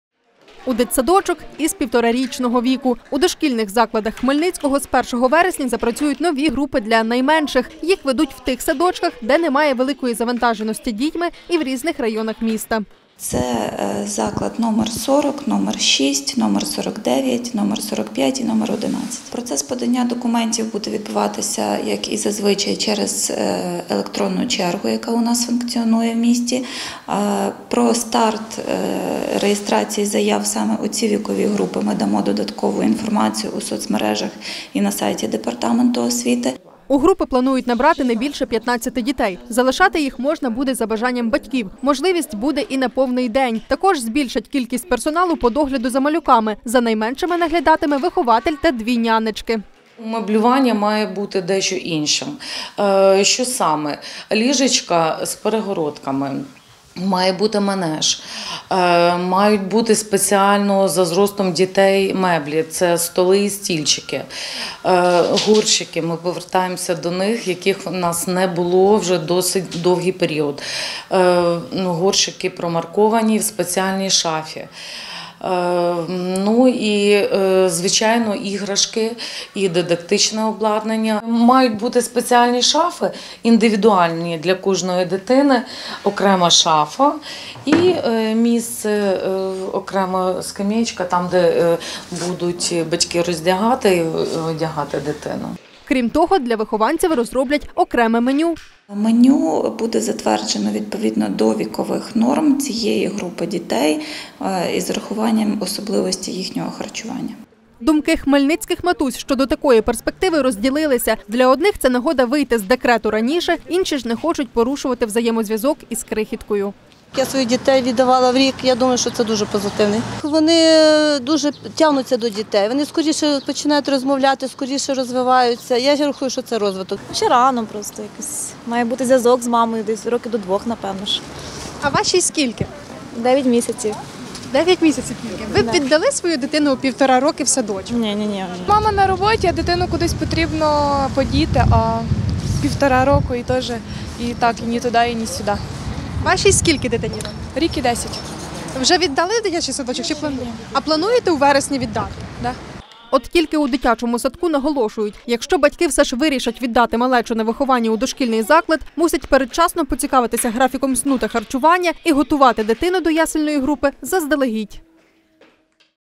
Ясельні групи запрацюють у хмельницьких дитсадочках. Перший Подільський 31.05.2021
З нового навчального року у 5 дитсадках Хмельницького введуть групи для півторарічних дітей. Для найменших розроблять окреме меню, а нагляд здійснюватиме дві нянечки і вихователь.  Чи готові батьки віддавати свою малечу до садочка з пелюшок – дивіться у сюжеті.